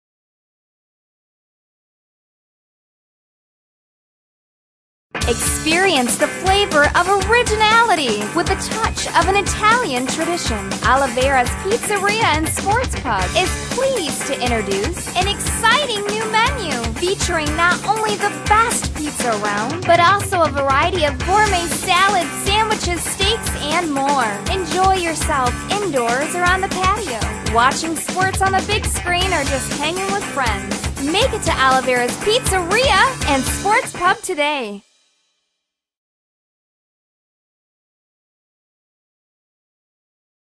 Olivera's Sports Pub
Located in Ionia, Michigan Olivera's Sports Pub has the best in Italian fare you can find anywhere. Whether you are looking for a slice at lunch time or a glass of fine wine and some great Italian food shared with family and friends, Olivera's Sports Pub is ready to serve you!